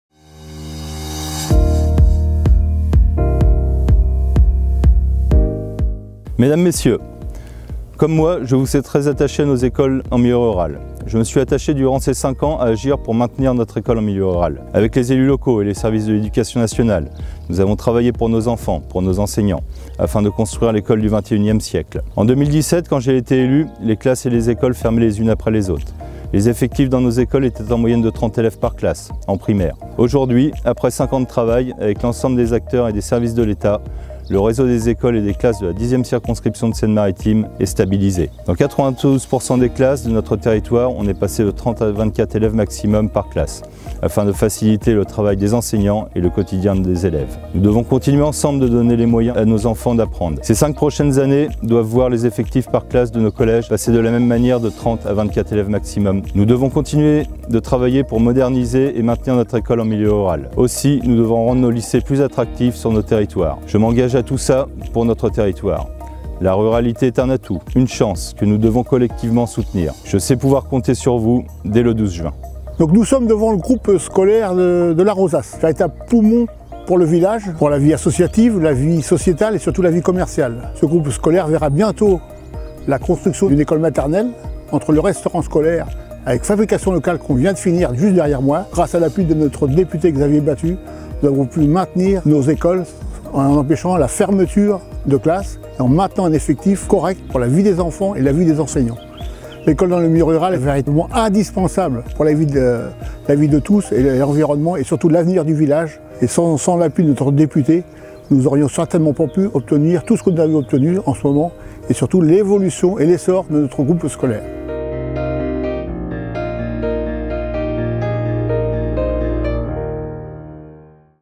Nos écoles : mes engagements pour les 5 années à venir :
Le député Xavier Batut, candidat à sa propre succession lors des élections legislatives 2022 pour la 10e circonscription de la Seine-Maritime, dévoile ses ambitions pour l'éducation en milieu rural.